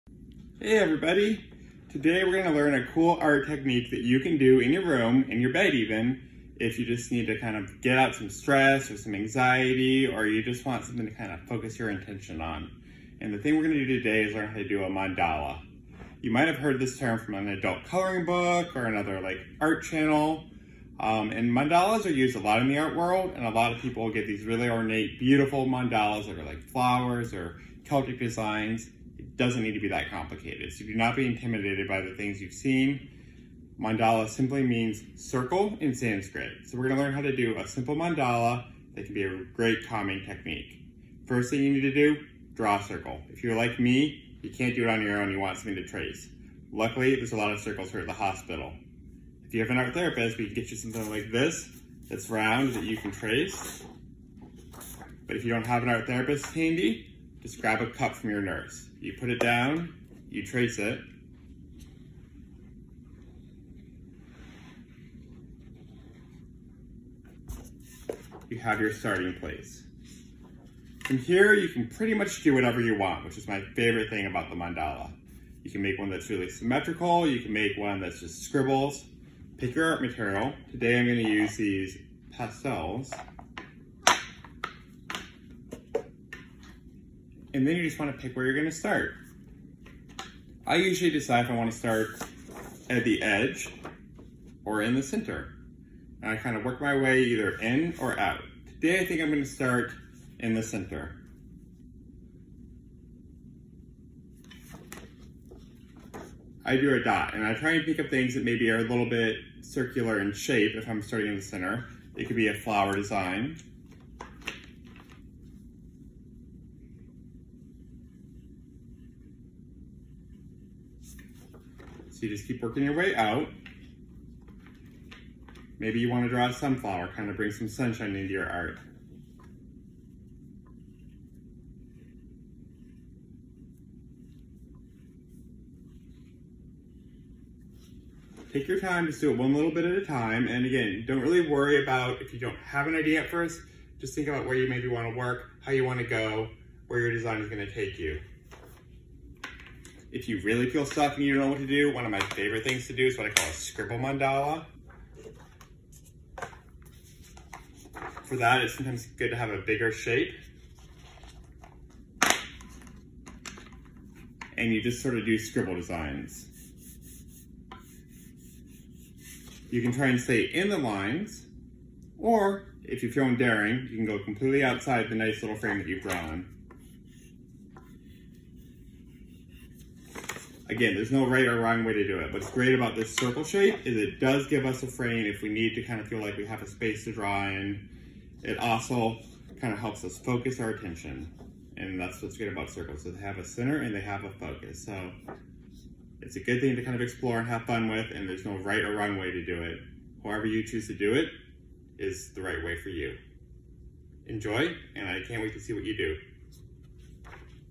Learn to Make Mandalas with Art Therapist Mike
Join Seattle Children's Art Therapist Mike Willen in learning how to create a mandala. You'll need a pencil or pen, art materials of your choosing (markers, color pencils, paint, or pastels) and a piece of paper.

Learn how making mandalas can help you relax and focus your attention while making art.

To learn more about Seattle Children's Art Therapy and Child Life Specialists, go